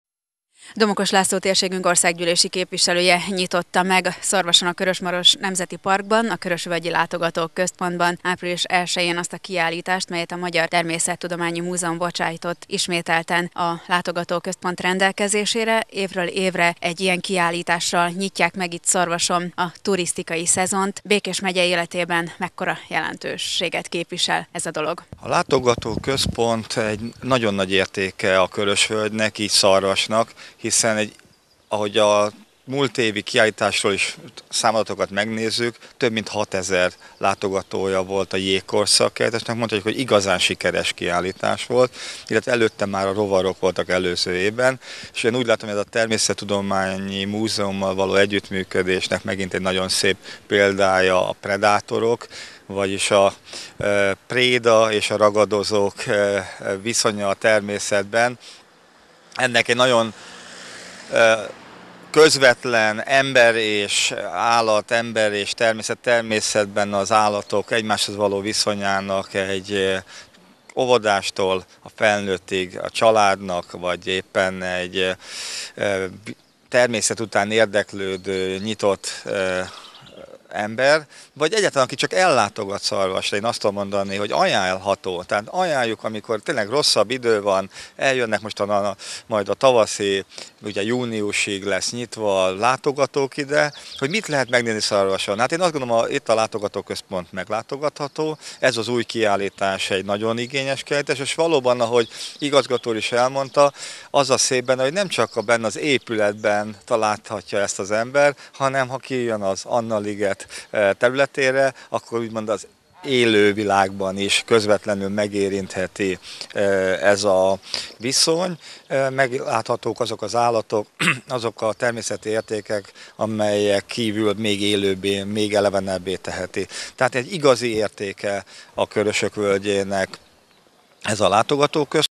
Domokos László a Körösvölgyi Látogatóközpontban
A ragadozó emlősök, hüllők és pókok legkülönfélébb zsákmányszerzési technikáit és trükkjeit bemutató Predátorok kiállítás április 1-jétől várja az érdeklődőket Szarvason, a Körös-Maros Nemzeti Park Körösvölgyi Látogatóközpontjában. A rendezvényt Dr. Bajzáth Judit, Domokos László és Tirják László nyitották meg.

Forrás: Körös TV